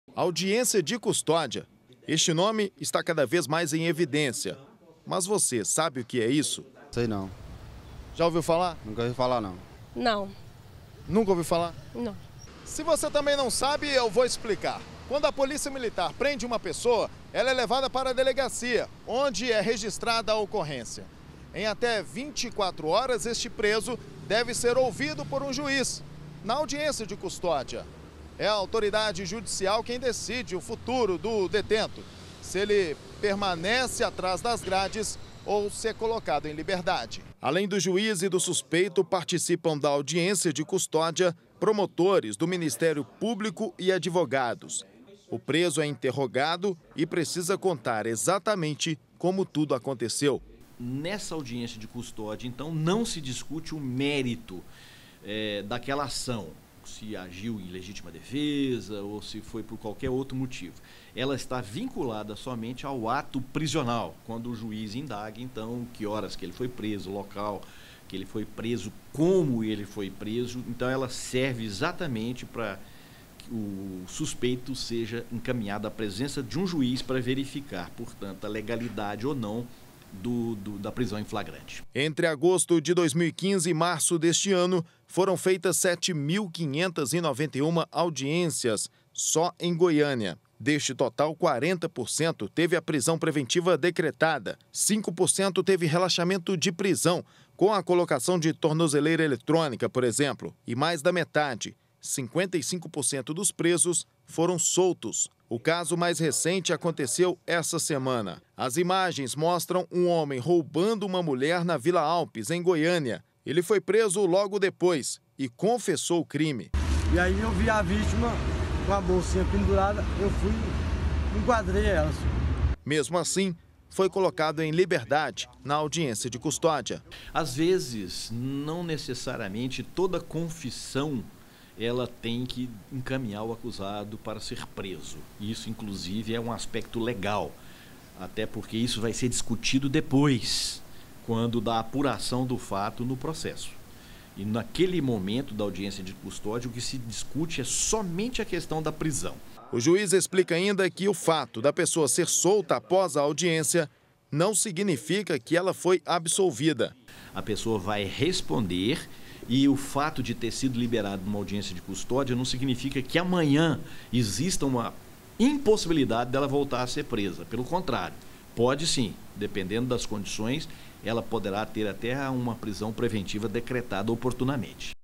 CA - Audiências de custódia: entenda como funciona - 29-08-2018
Presente no poder judiciário desde 2015, as audiências de custódia são polêmicas. Afinal frequentemente liberam suspeitos da prisão. Mas você sabe como funcionam estas audiências??